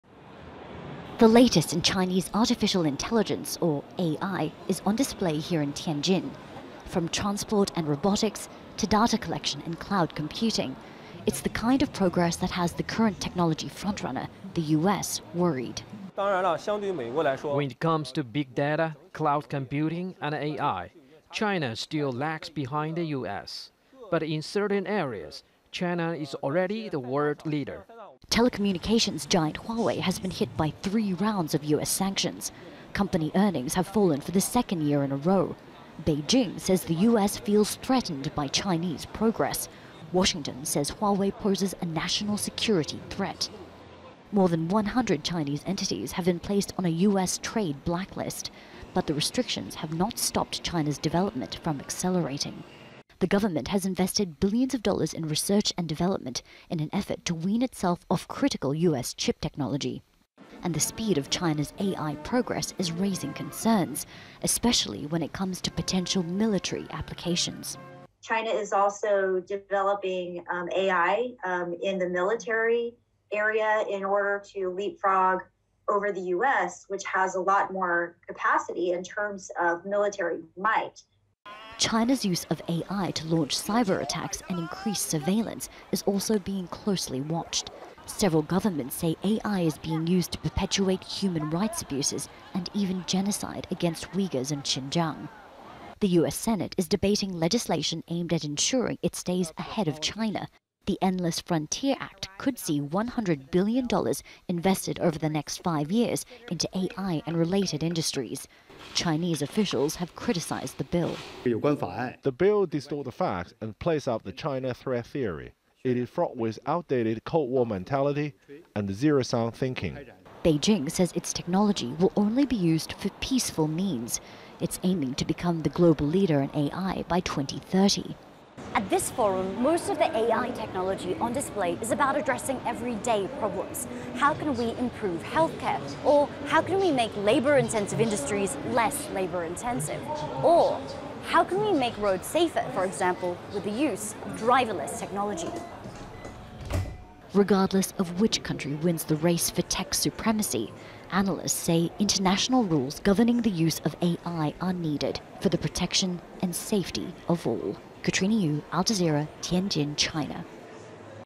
China showcases artificial intelligence advances
China is showcasing its advances in the field of artificial intelligence - at an annual tech-themed forum.
It is being held in the port city of Tianjin.
Super computers, rockets and robots are among the innovations on display.
The event comes just months after the United States National Security Commission said that China is on track to overtake the US as the world's AI superpower.
Al Jazeera's Katrina Yu reports.